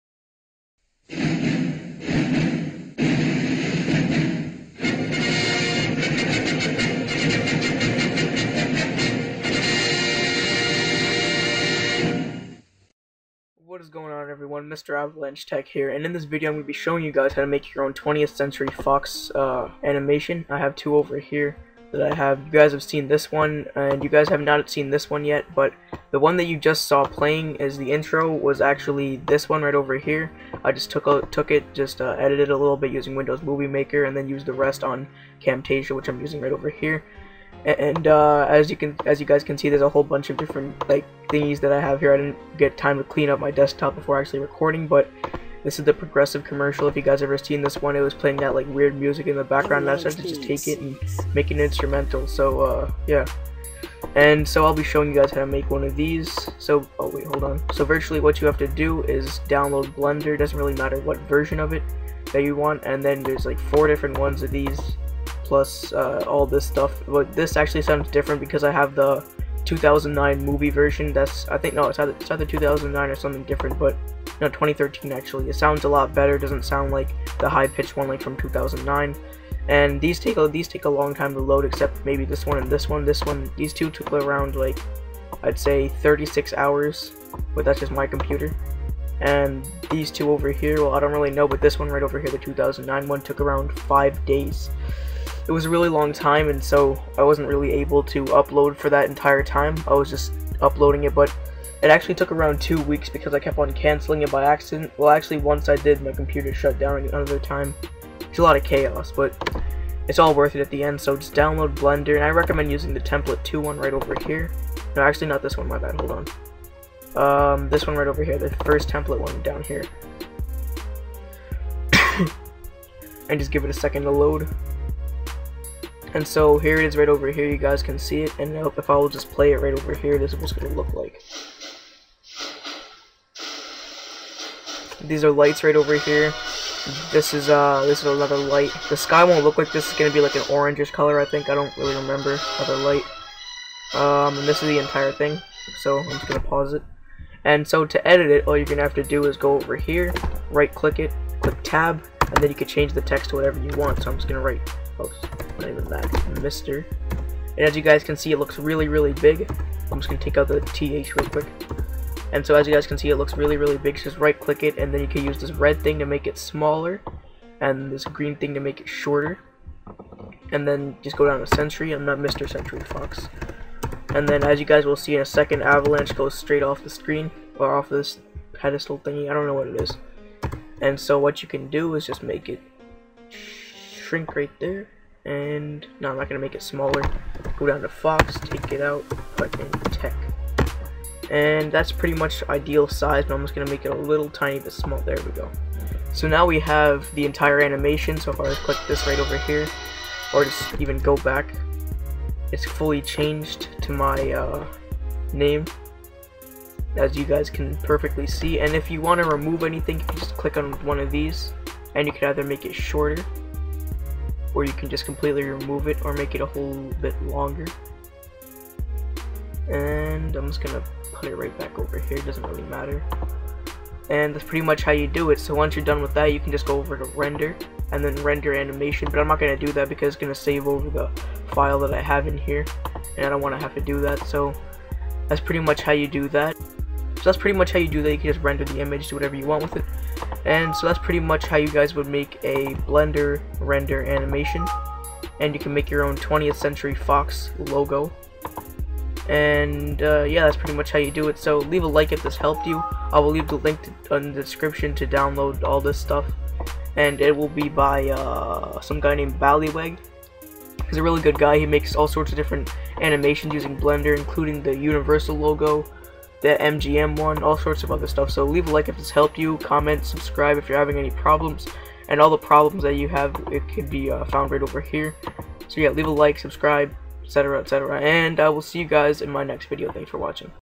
Make your own 20th Century Fox Logo using Blender
READ:
This is how to make your own 20th Century Fox Anime using your own details. You can make it say anything you want! The one seen first is edited with WMM and Camtasia.